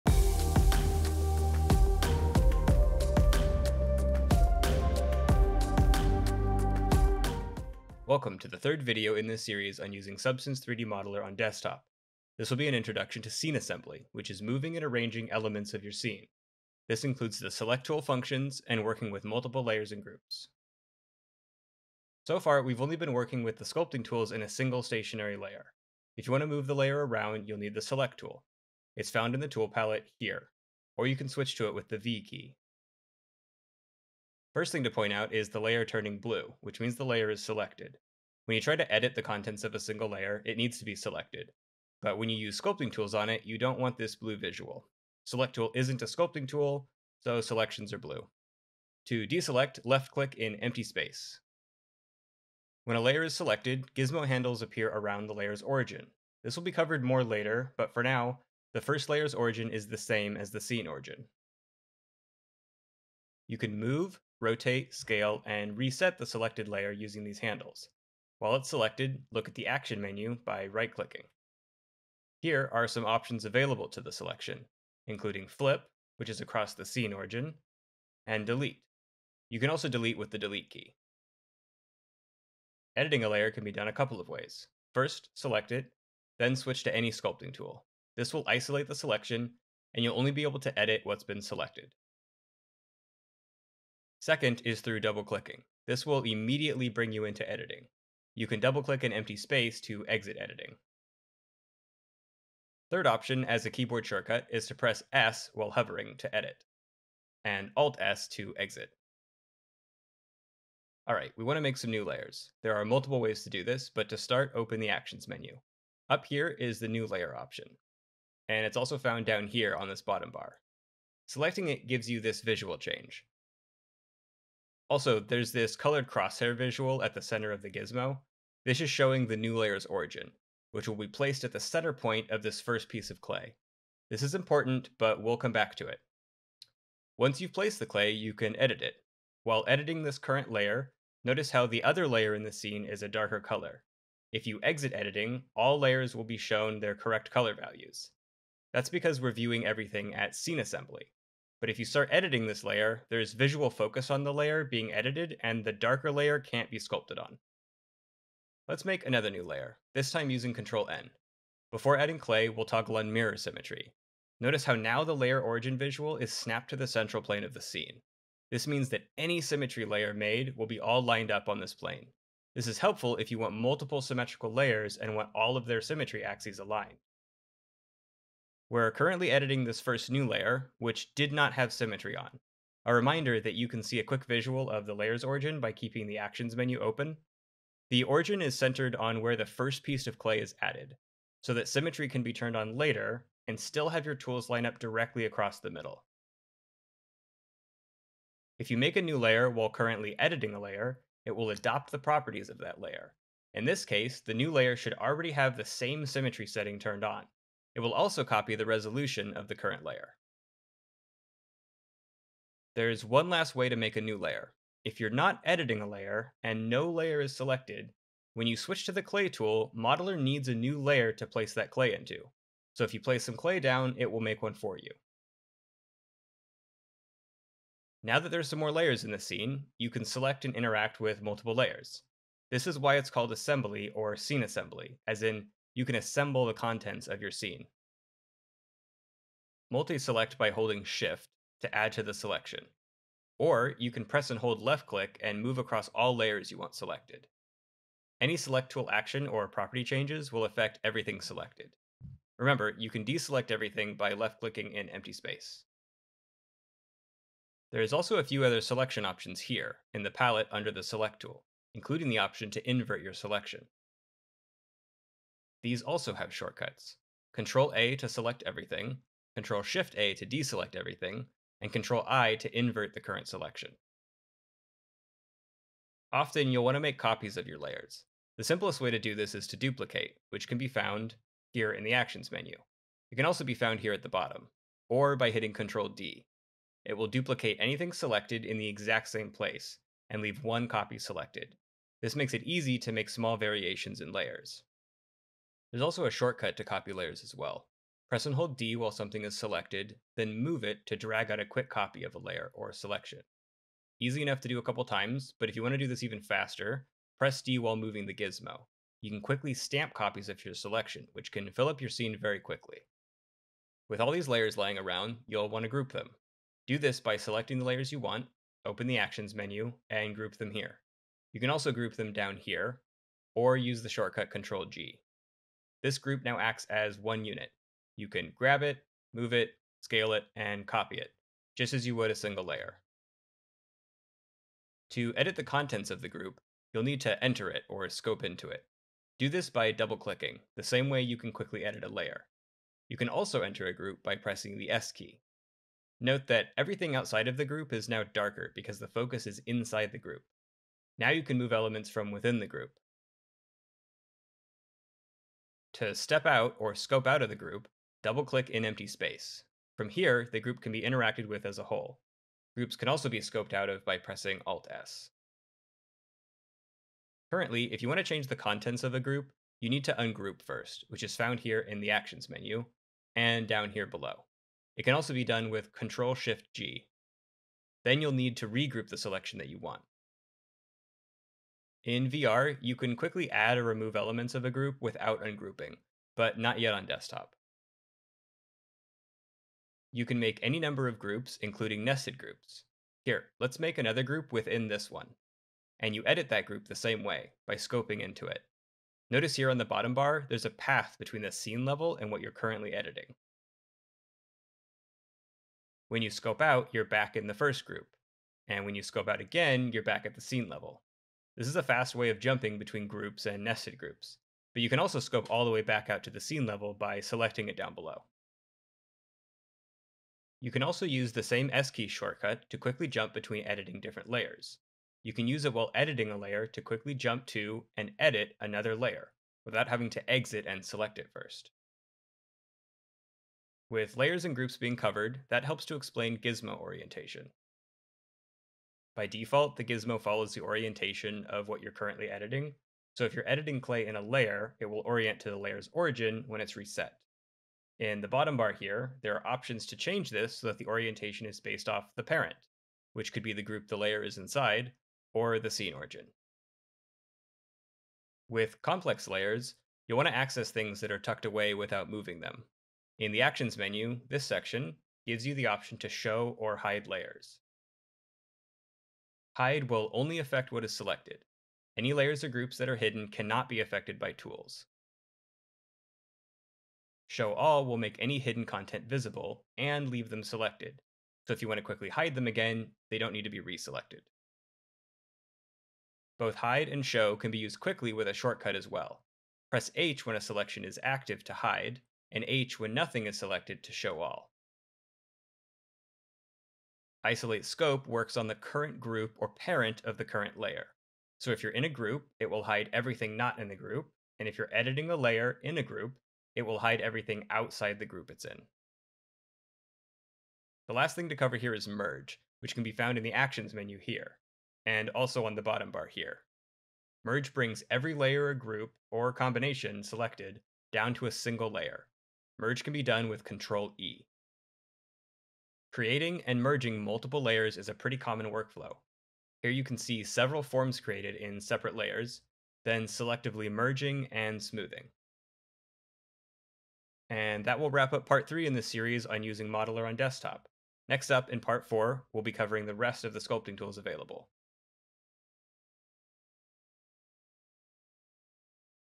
First Steps with Substance 3D Modeler - 03 Assembly (Desktop Mode) | Adobe Substance 3D
In this video for Substance 3D Modeler, we'll cover assembly for Desktop.